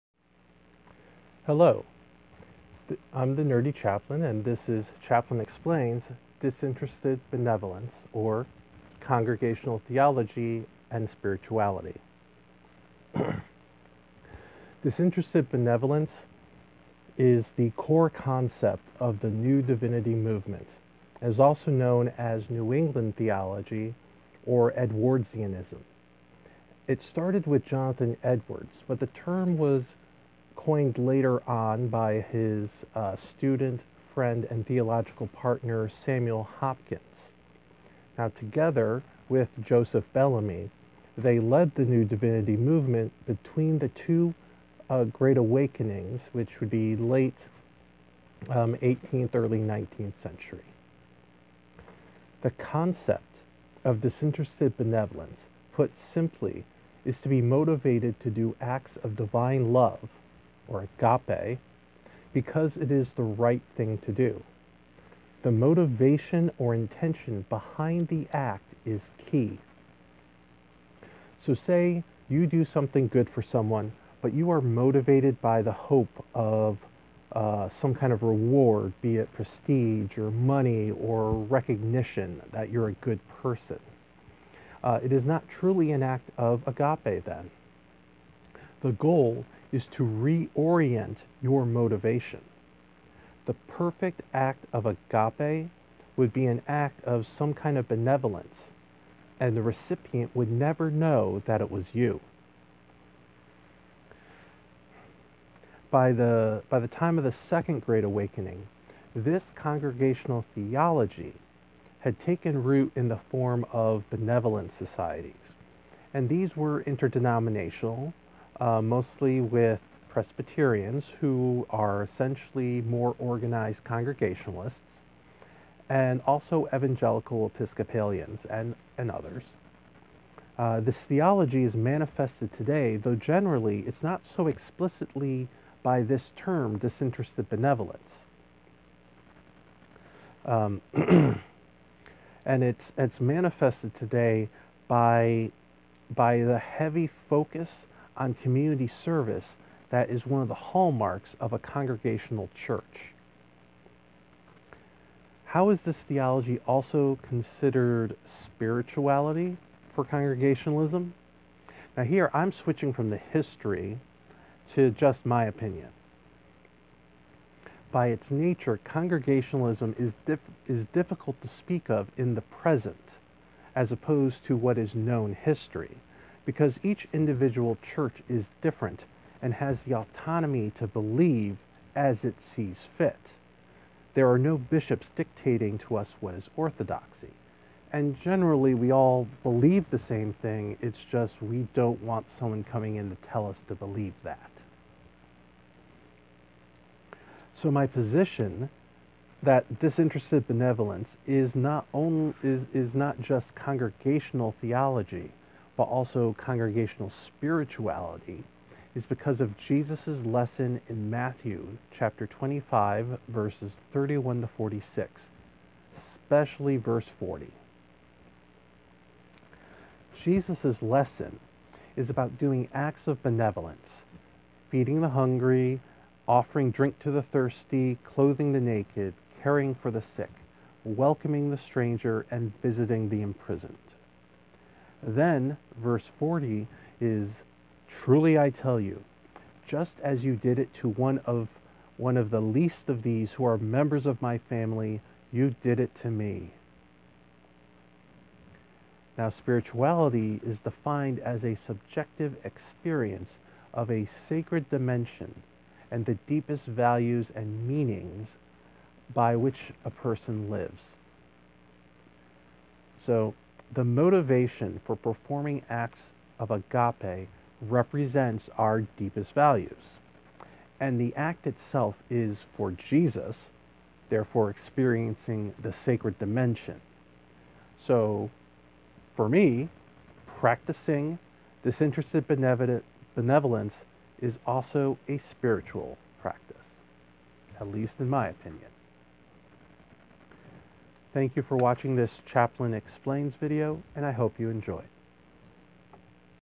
Interfaith Chaplain Explains: Disinterested Benevolence (Congregational Theology & Spirituality)
Chaplain Explains Disinterested Benevolence, the basis of Congregational Theology and Spirituality, originating with Congregational theologian Jonathan Edwards, and popularized by his students/theological partners Joseph Bellamy & Samuel Hopkins, the three together led the New Divinity Movement between the two Great Awakenings.